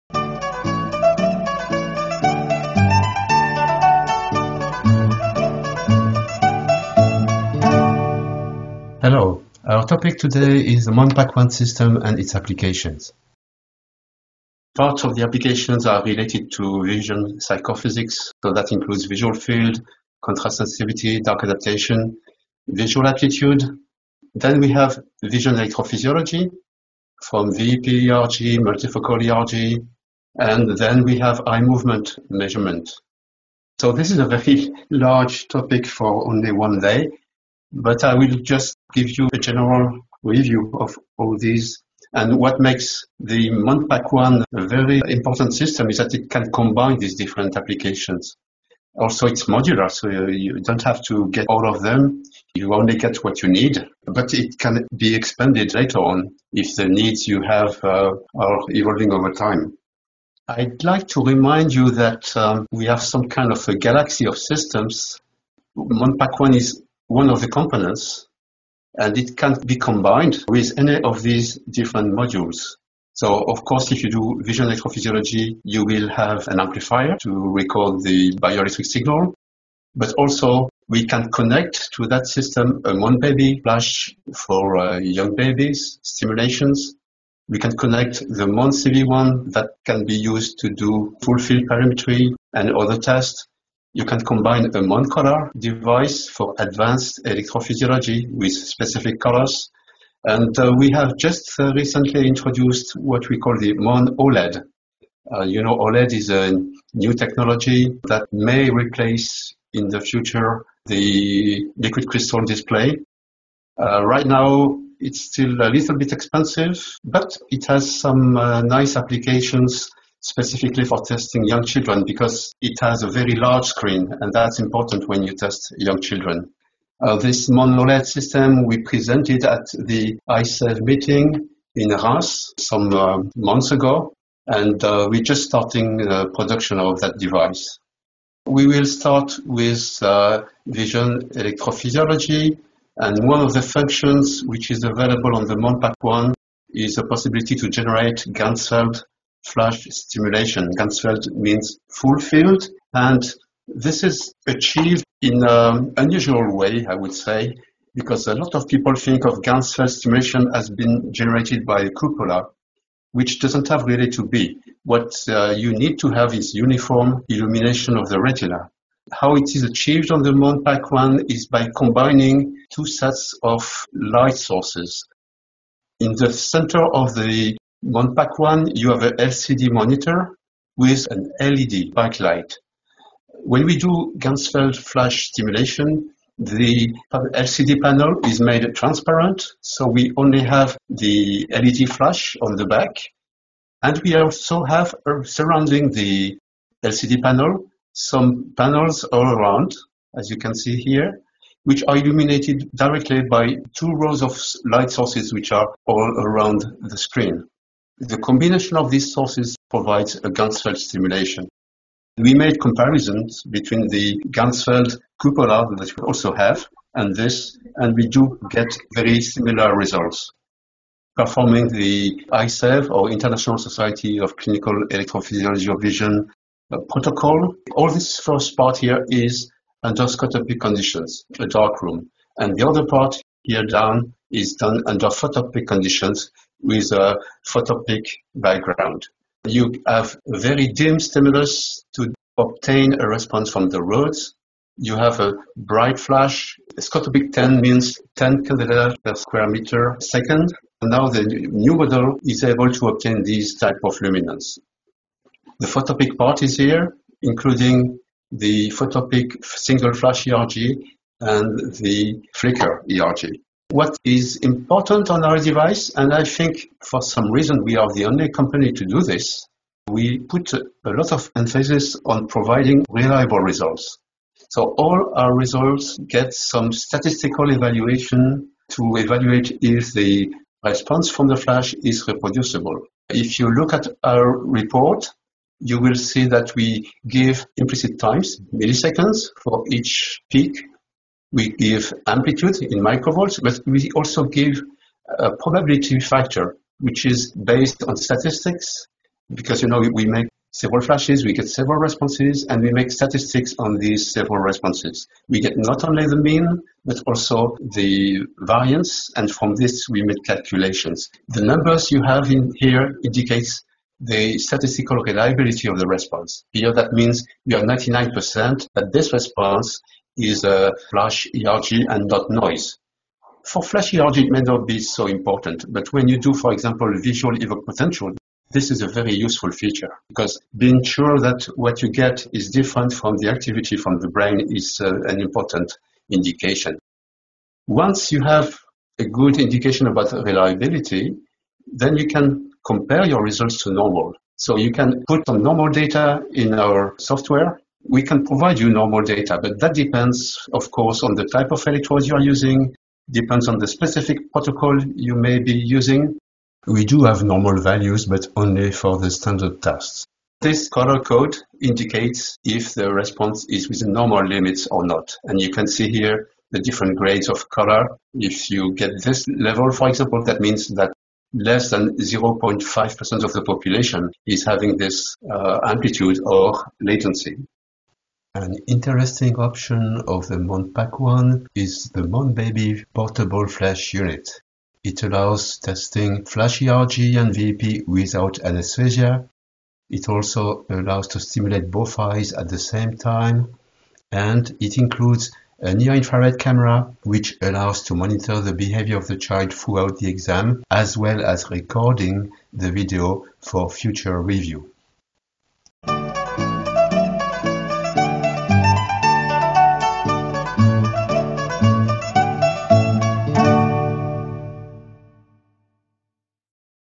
MonPackONE -1- Vision electrophysiology - Ganzfeld flash ERG
This video is the first of a series describing the MonPackONE system, a compact, multifunction system for visual function evaluation manufactured by Metrovision of France.  It allows the realization of vision psychophysics, vision electrophysiology and eye movement tests.
In this first video, we will describe how ganzfeld flash electroretinography exams are realized.